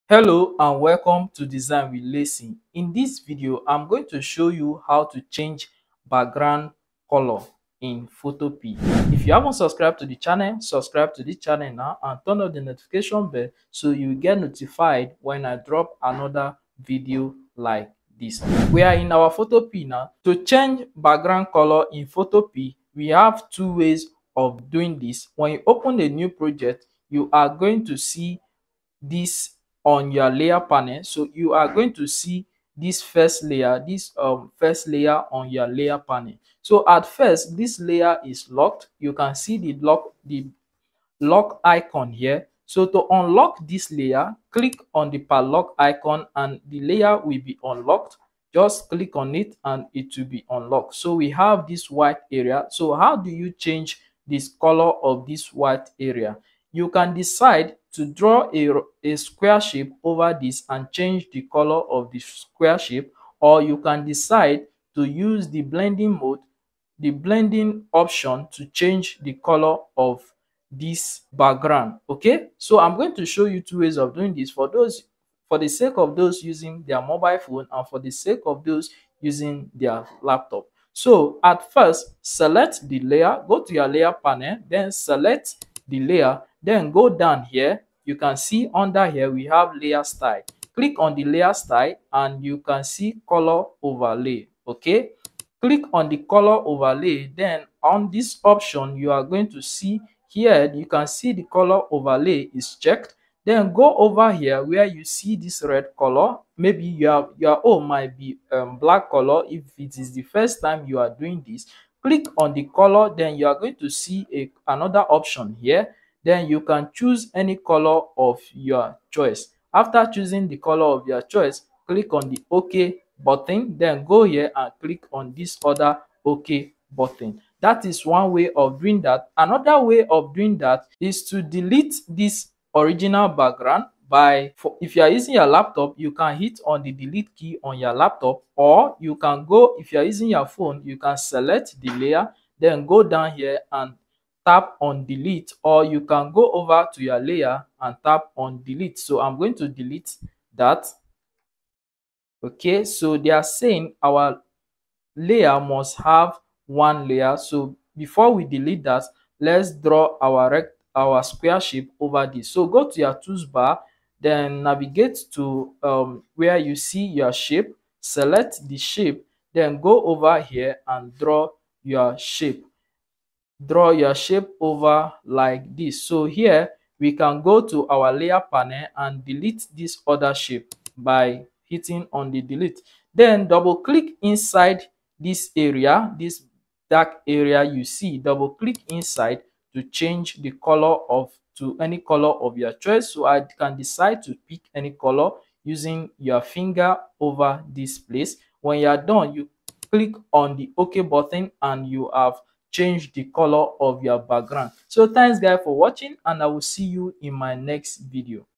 How To Change Background Color in PhotoPea | Photopea 2023
Learn how to change the background color of any image in Photopea, a free online image editor that is similar to Photoshop. This tutorial is easy to follow and perfect for beginners.